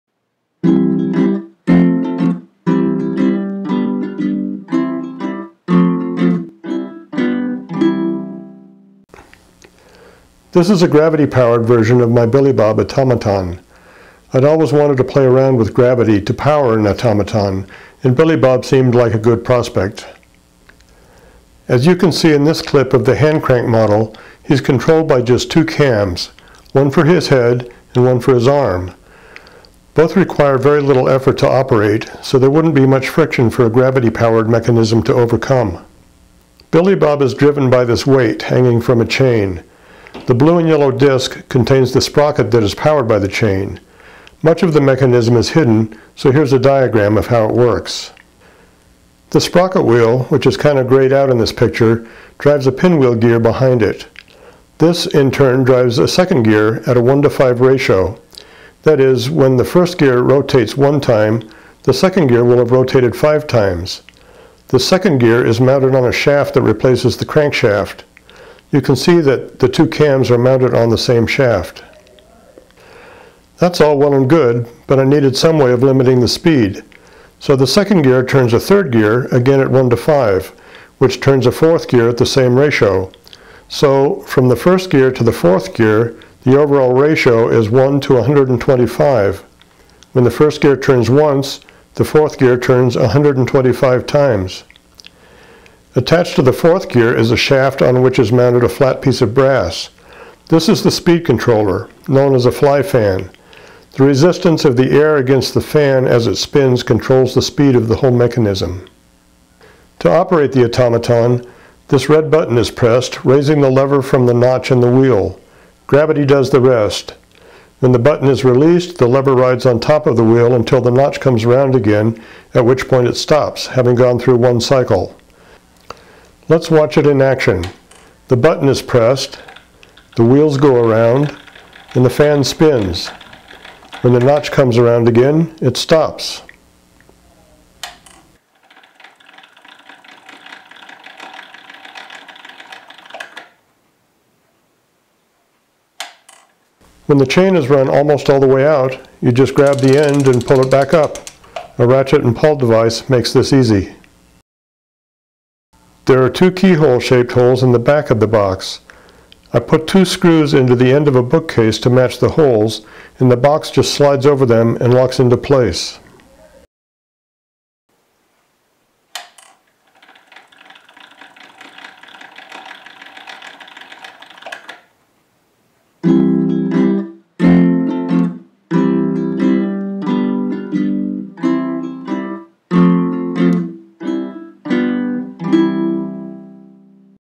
Billy Bob (gravity-powered version)
This video describes how I converted a hand-cranked automaton to a gravity-powered version.